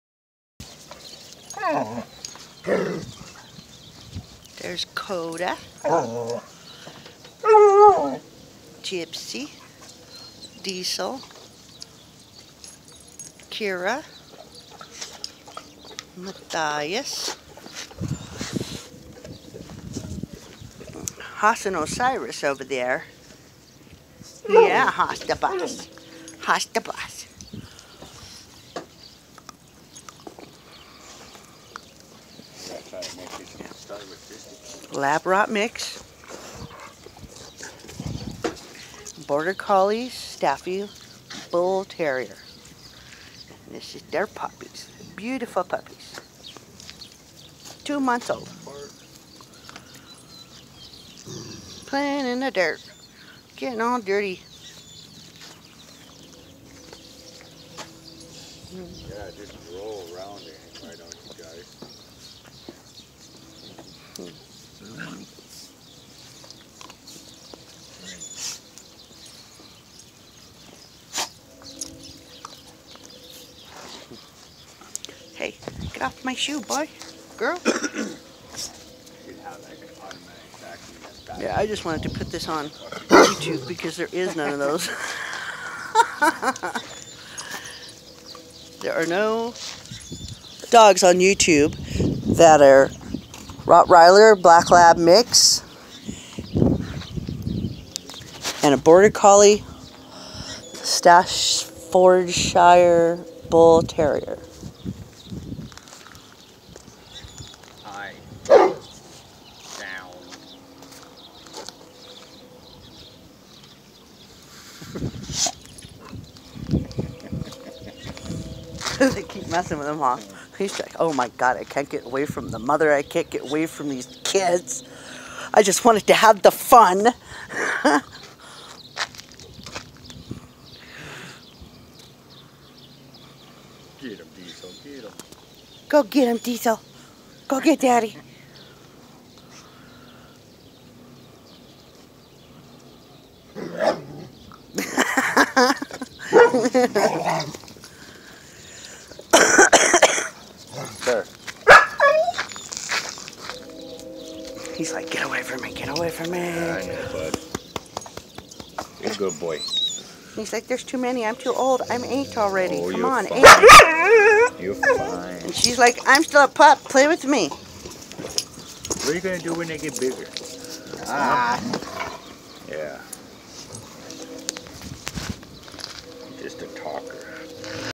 rottie/ black lab + border collie/staffordshire bull terrier mix puppies
puppies are 2 months old. Father of puppies is a rottie /lab mix and mother is a border collie/staffordshire bull terrier  mix.
together they make beautiful puppies